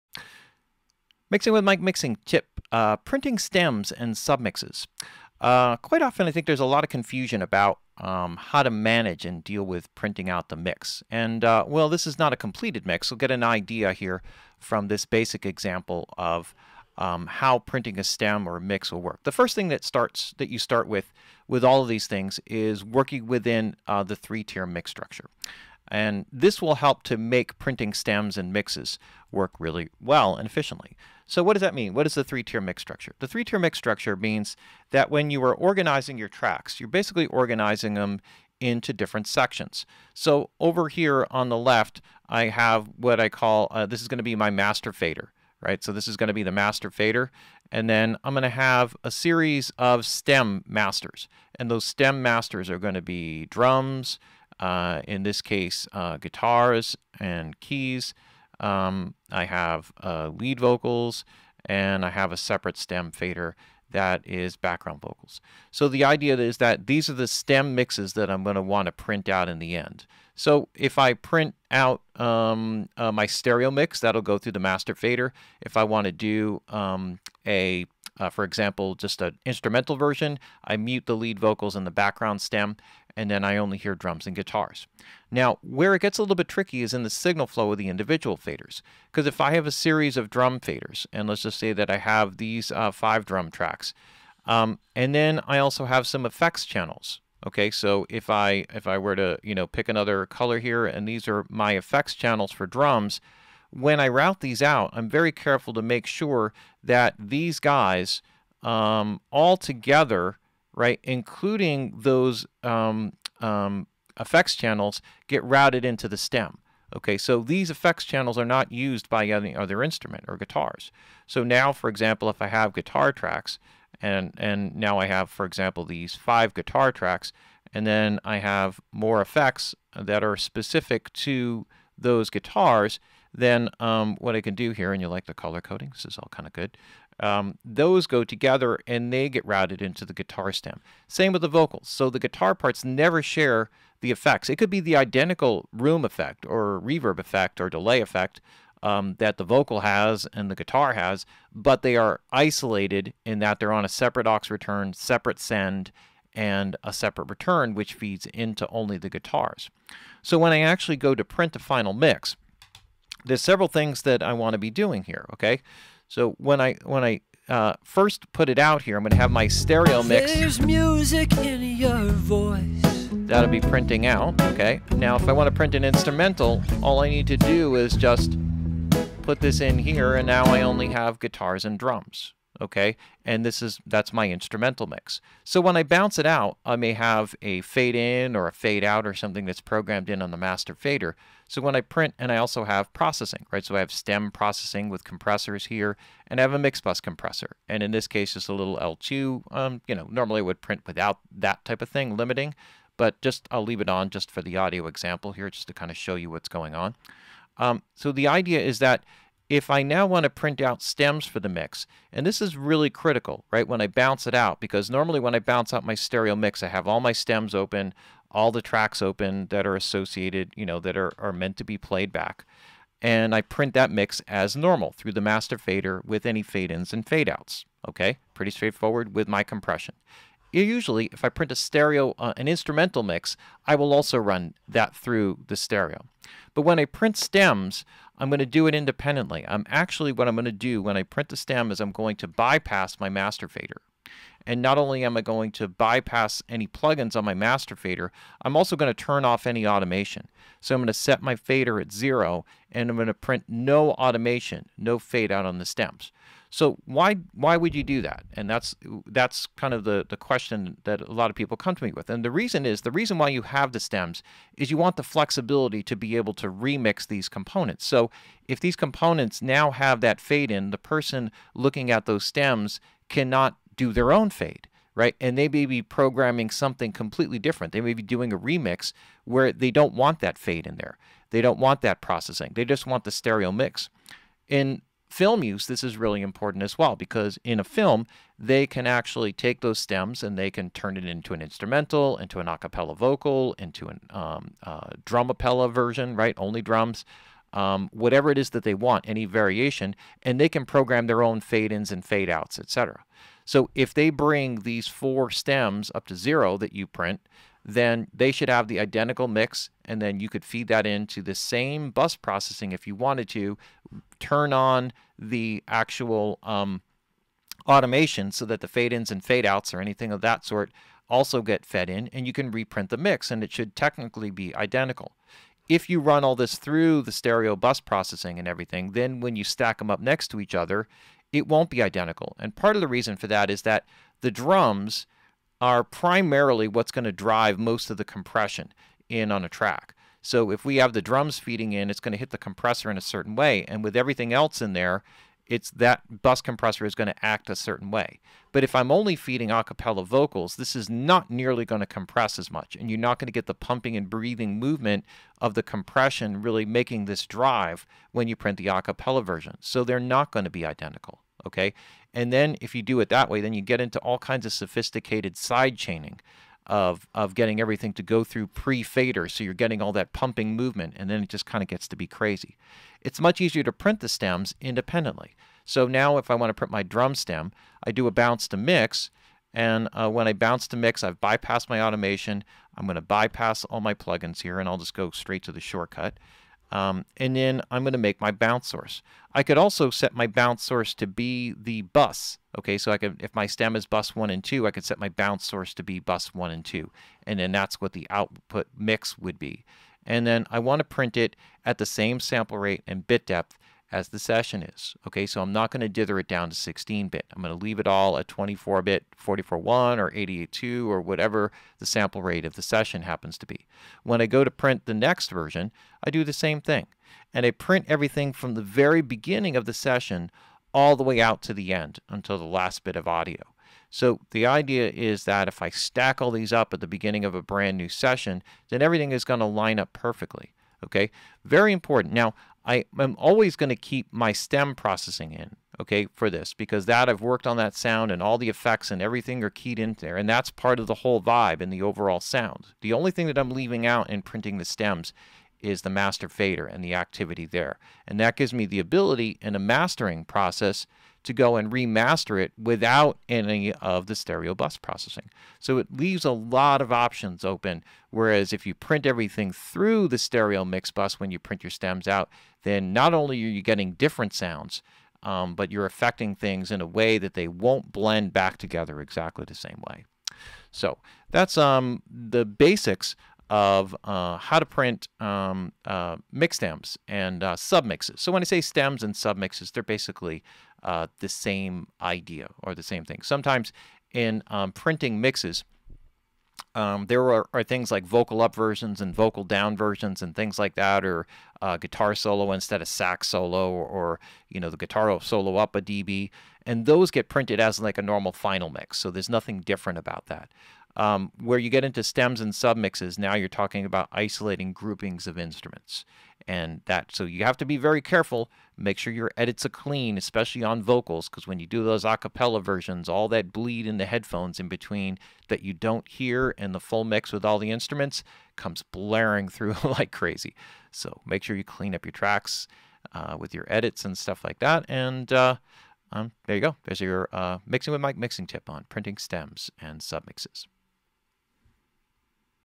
Mixing With Mike Mixing Tip: Printing Stems and Sub Mixes
This video is from the "Mixing With Mike" live online classes every Wednesday evening @ 4PM US Pacific time. All live classes may be attended for Free!